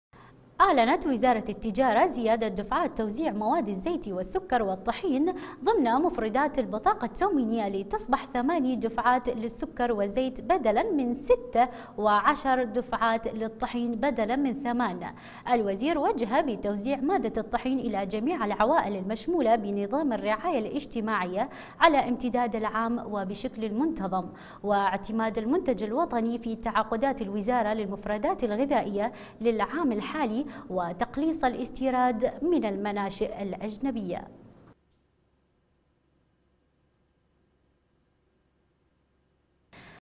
البطاقة التموينية زيادة دفعات توزيع الزيت والسكر ‏والطحين
البطاقة التموينية
زيادة دفعات توزيع الزيت والسكر ‏والطحين

اعلنت وزارة التجارة زيادة دفعات توزيع مواد الزيت والسكر ‏والطحين ضمن مفردات البطاقة التموينية لتصبح ثماني دفعات للسكر والزيت بدلا ‏من ست دفعات وعشر دفعات للطحين بدلا من ثمان .‏ وذكرت الوزارة في بيان لها ان الوزير وجه بتوزيع مادة الطحين الى جميع العوائل ‏المشمولة بنظام الرعاية الاجتماعية على امتداد العام وبشكل منتظم.‏ مشيرة الى اعتماد المنتج الوطني في تعاقدات الوزارة للمفردات الغذائية للعام الحالي ‏وتقليص الاستيراد من المناشئ الاجنبية. ‏